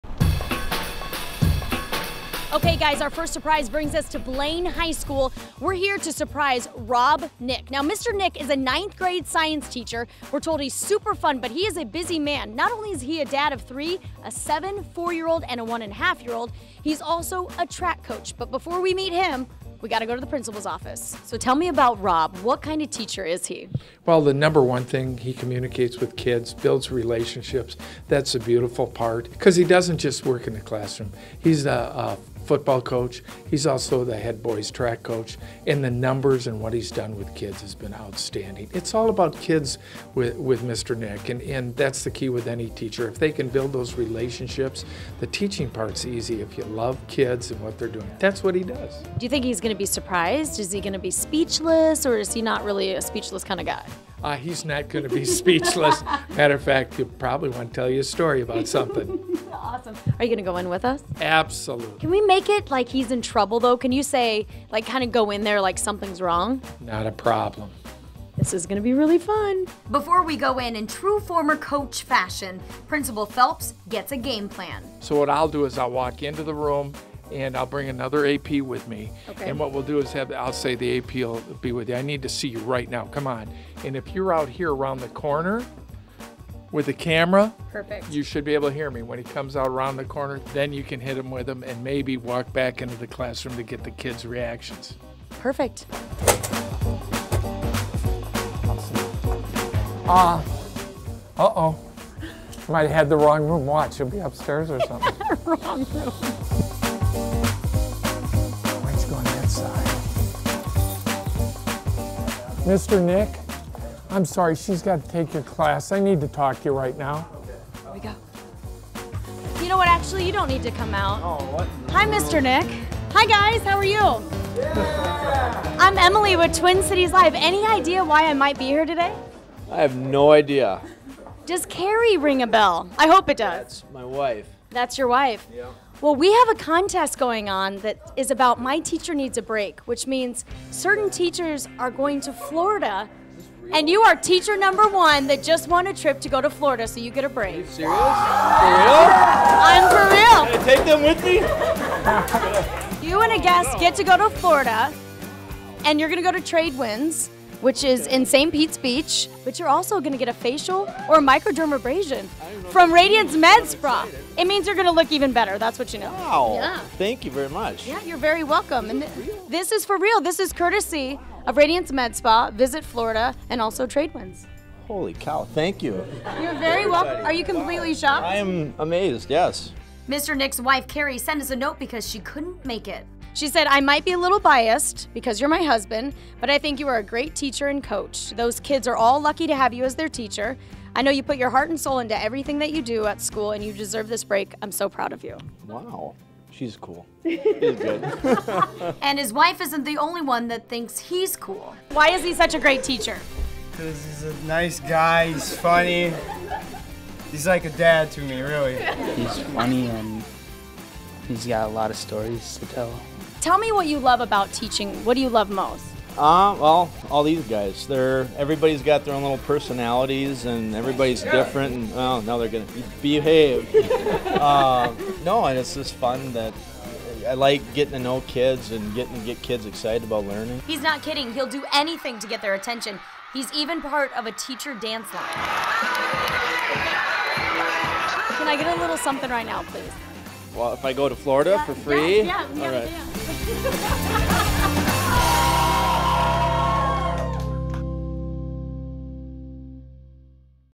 "My Teacher Needs a Break" Florida Giveaway #1
TCL Reporter Emily Engberg surprises Rob Nick, a teacher at Blaine High School with spa package and a trip to Florida, courtesy of Radiance MedSpa and VisitFlorida.com